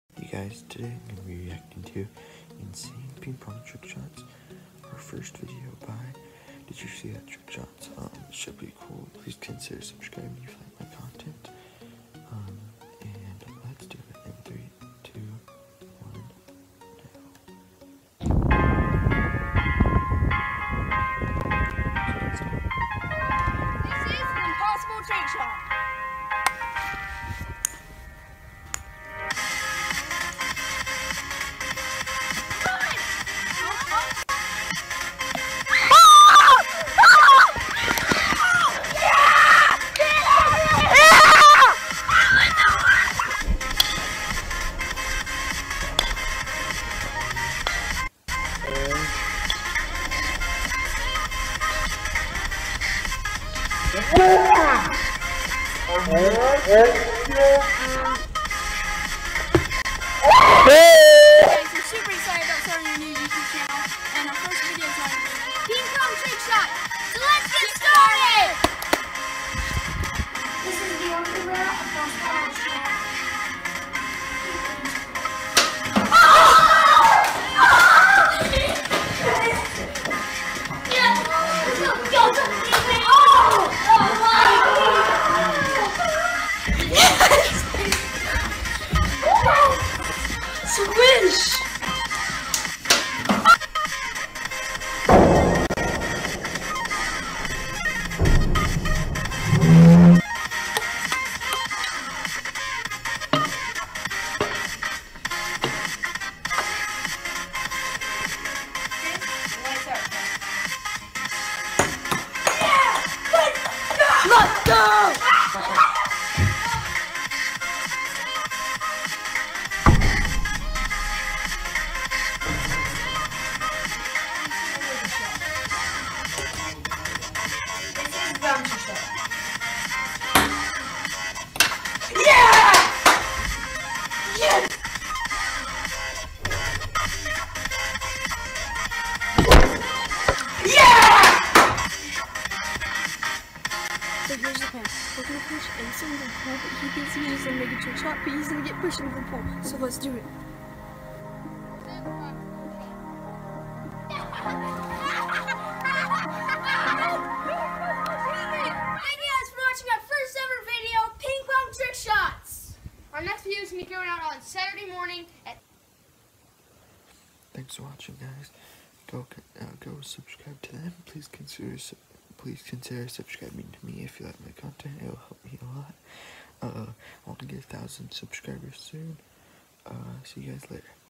Reacting To Insane Ping Pong Trick Shots By Did You See That-Trick Shots
Here is the link to Did you see that -Trick Shots